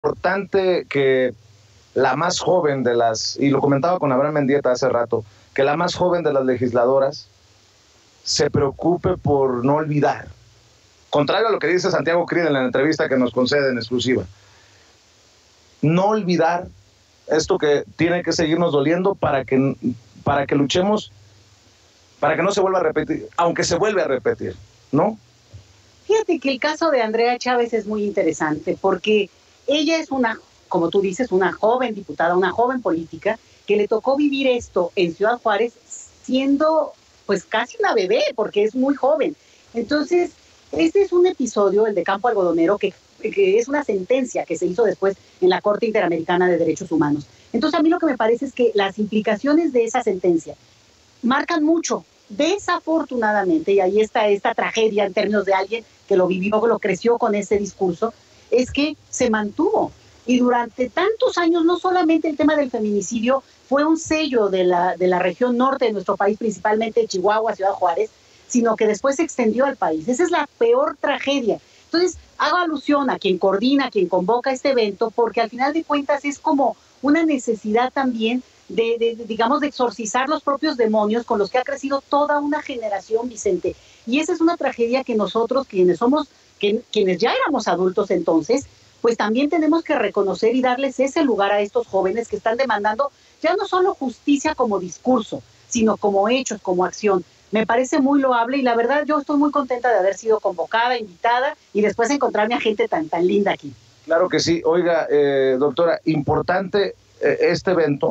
AL FINAL EL FEMINICIDIO SE EXTENDIÓ A TODO EL PAÍS: DRA. CALDERÓN CHELIUS
La diputada Andrea Chávez llama a no olvidar para que no vuelva a ocurrir la violencia contra las mujeres. La doctora Leticia Calderón Chelius nos comenta que se mantuvo durante muchos años el feminicidio como un sello en la región norte del país; y que la peor tragedia es que al final se extendió a todo el país.